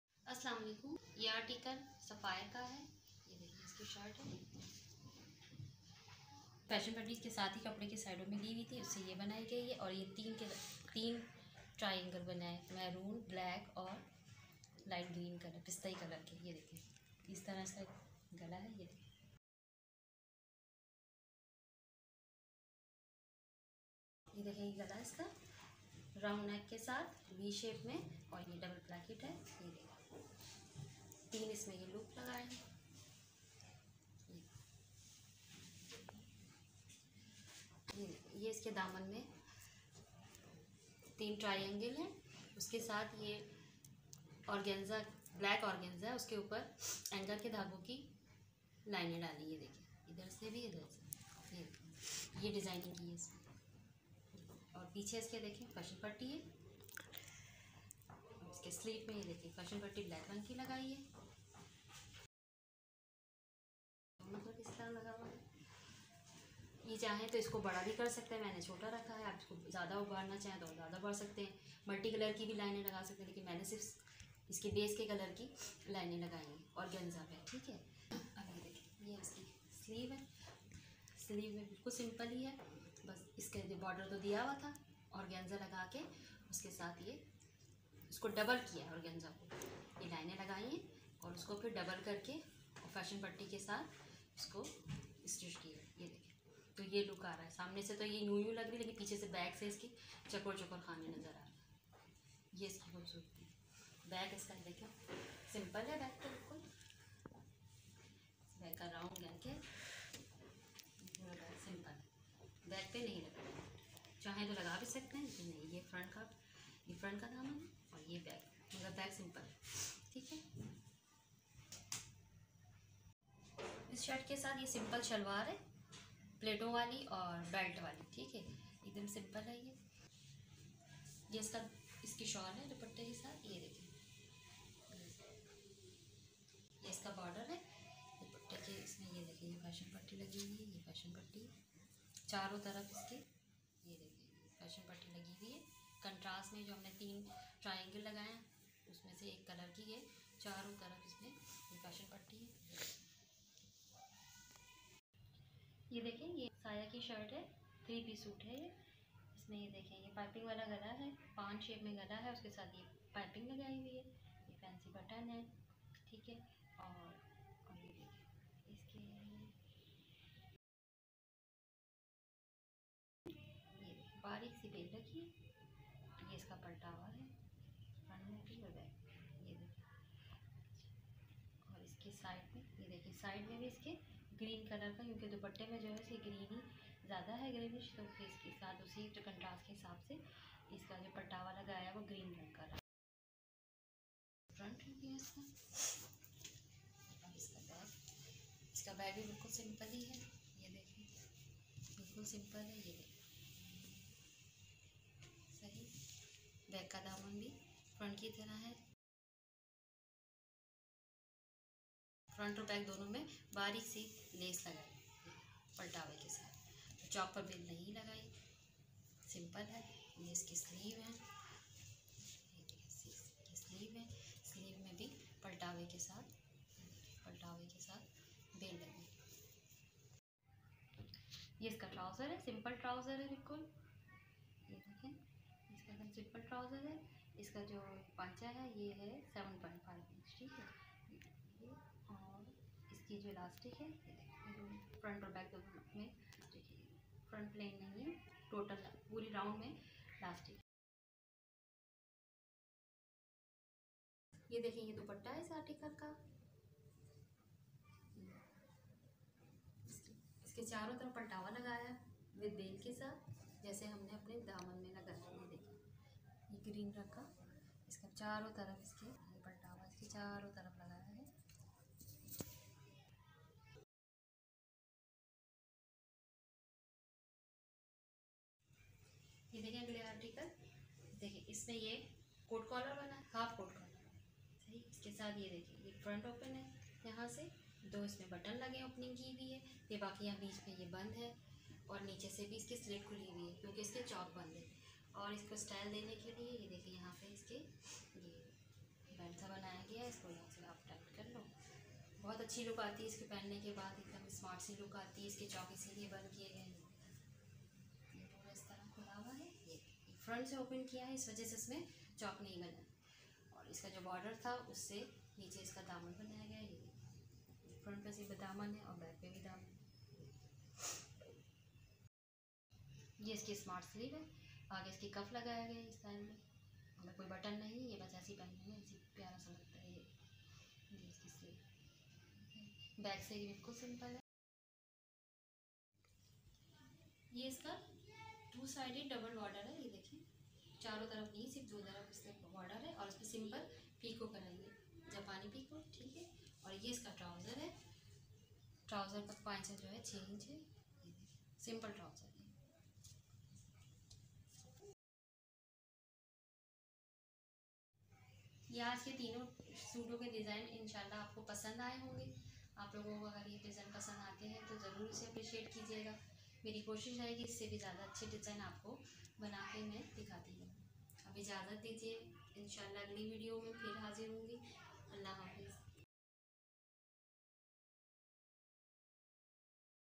Different neck and sleeves designs|Beautiful organza design
@stylishwear1107 provide you a complete video of different designs of neck, sleeves and Daman with fashion patti